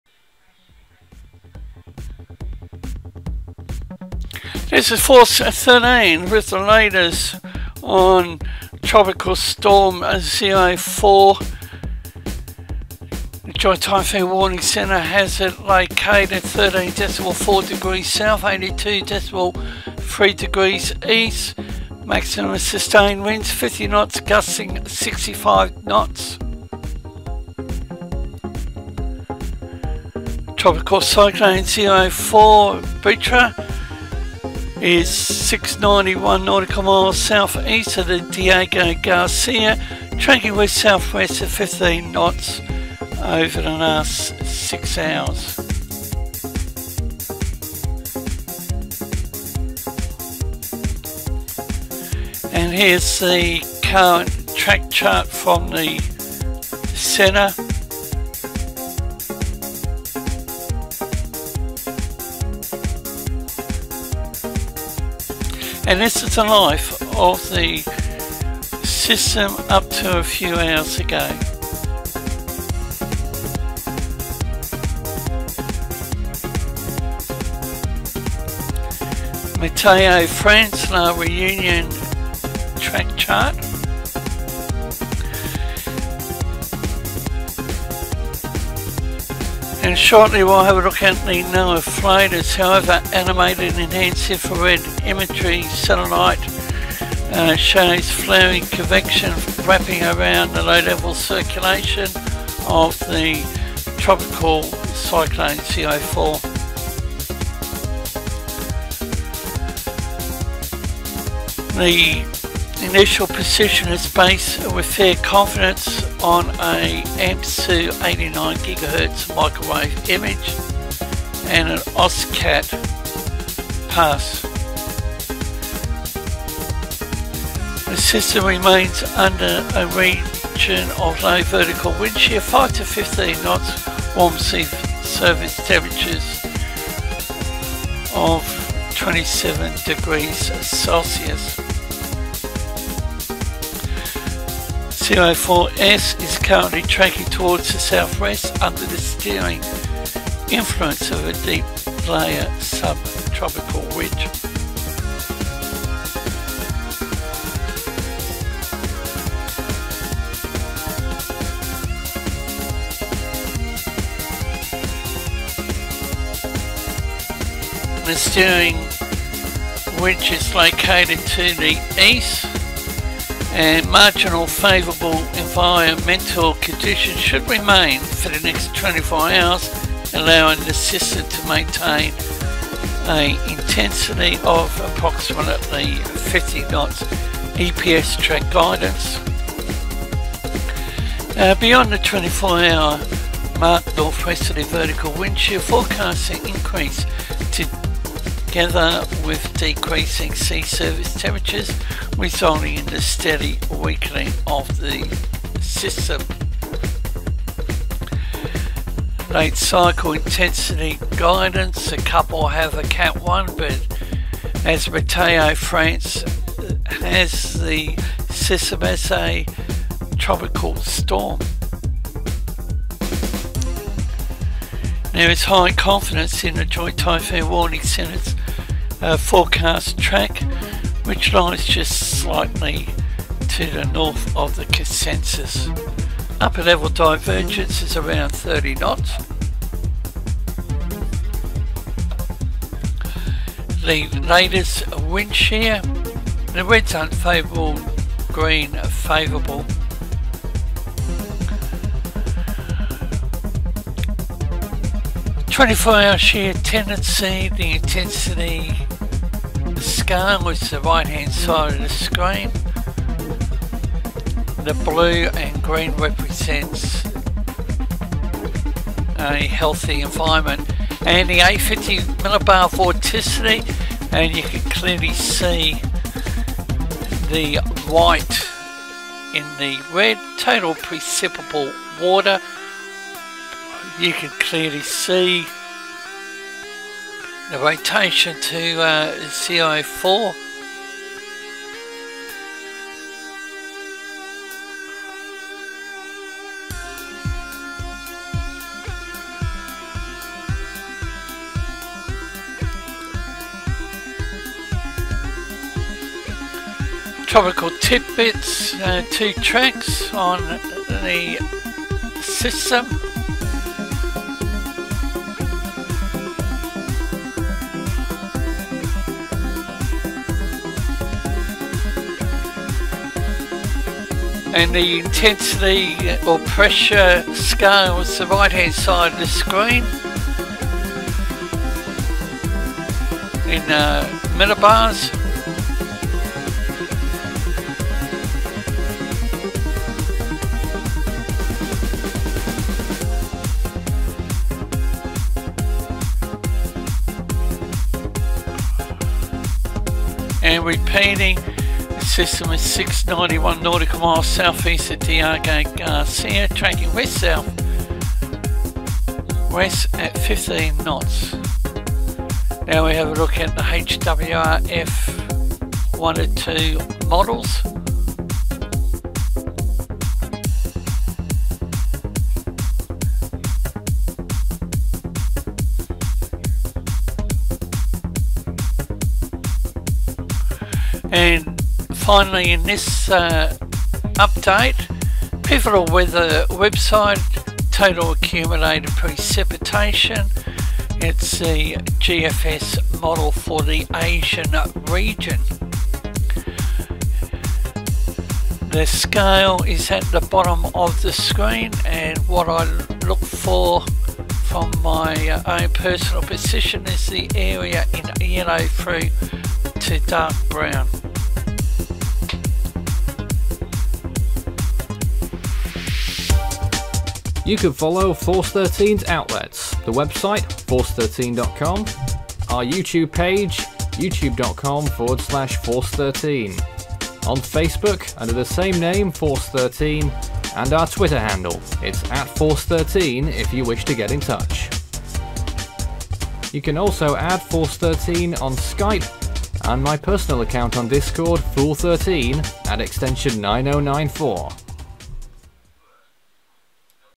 Latest on Tropical Storm (04) Bouchra November 19 05:52Hrs
181800Z --- NEAR 13.4S 82.3E
     MOVEMENT PAST SIX HOURS - 245 DEGREES AT 15 KTS
     POSITION ACCURATE TO WITHIN 060 NM
     POSITION BASED ON CENTER LOCATED BY SATELLITE
   PRESENT WIND DISTRIBUTION:
   MAX SUSTAINED WINDS - 050 KT, GUSTS 065 KTS.

TROPICAL CYCLONE 04S (BOUCHRA), LOCATED APPROXIMATELY 691 NM SOUTHEAST OF DIEGO GARCIA, HAS TRACKED WEST-SOUTHWESTWARD AT 15 KNOTS OVER THE PAST SIX HOURS.